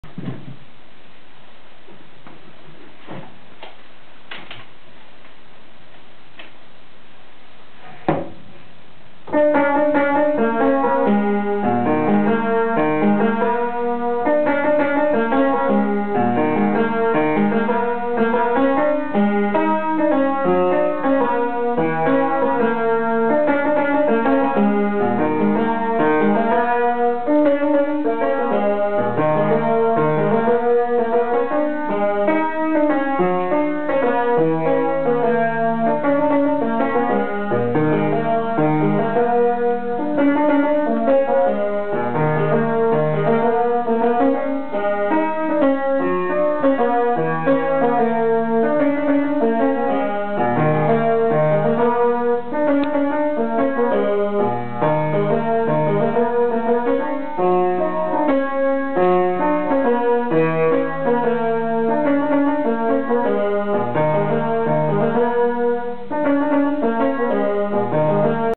For sale grand square piano made about 1835 for 600 euro
Playing my Brasilian rosewood grand square piano from 1835 for sale 600 euro plus your transportation from Riga, made by J Borchhardt.